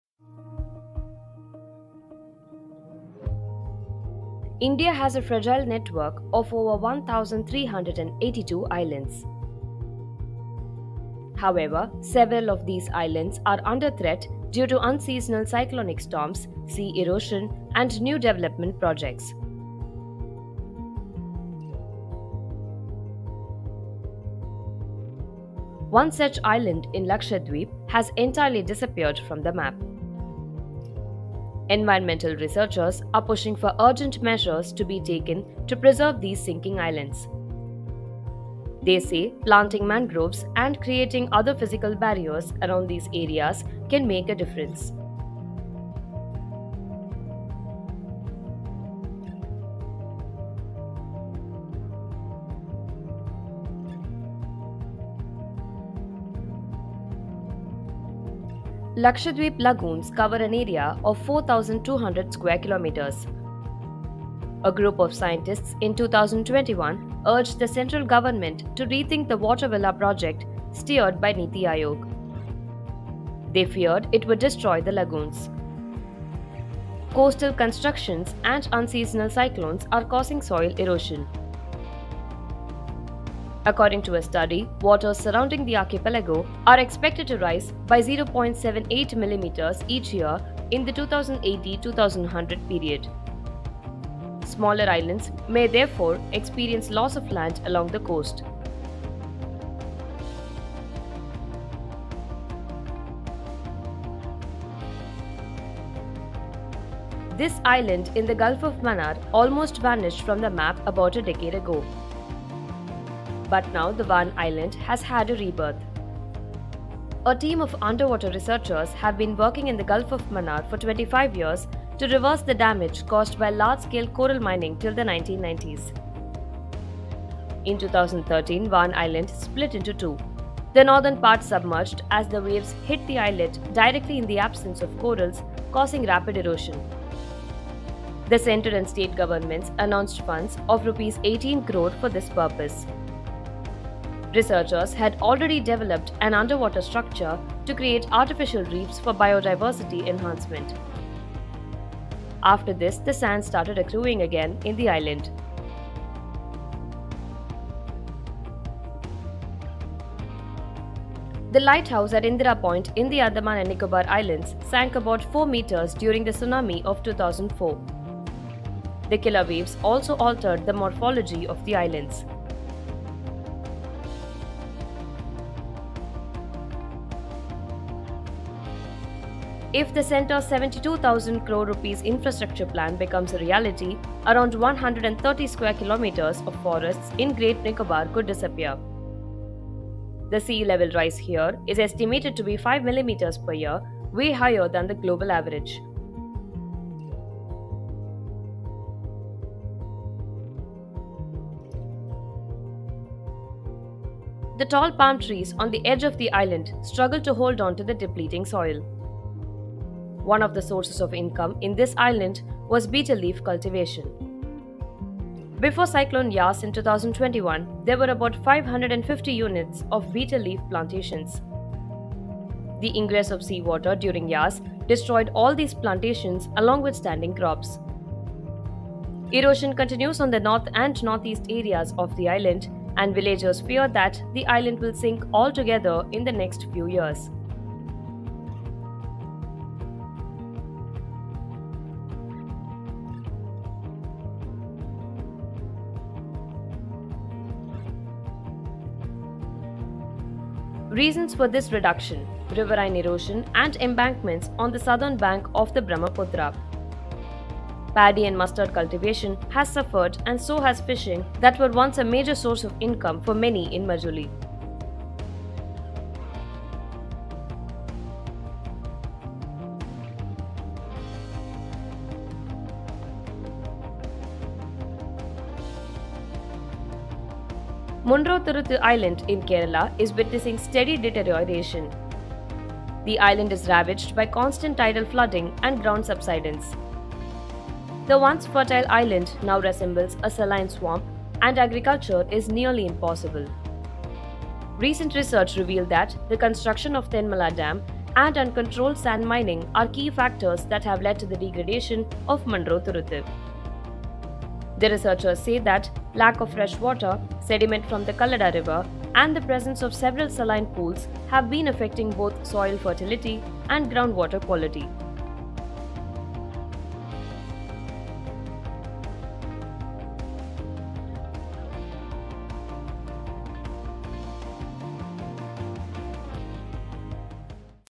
Why are some islands in India sinking? | The Hindu
India has a fragile network of over 1,382 islands. However, several of these islands are under threat due to unseasonal cyclonic storms, sea erosion and new development projects. One such island in Lakshadweep has entirely disappeared from the map.

Environmental researchers are pushing for urgent measures to be taken to preserve these sinking islands. They say planting mangroves and creating other physical barriers around these areas can make a difference.

Here's a look at islands across the country that are under threat.

Reporting: S. Anandan, B. Aravind Kumar, Shiv Sahay Singh, Rahul Karmakar, Navamy Sudhish

Voiceover & Production: Abhinaya Sriram

Photos & Videos: Ritu Raj Konwar, C. Sureshkumar, Debabish Bhaduri, N Rajesh, Getty Images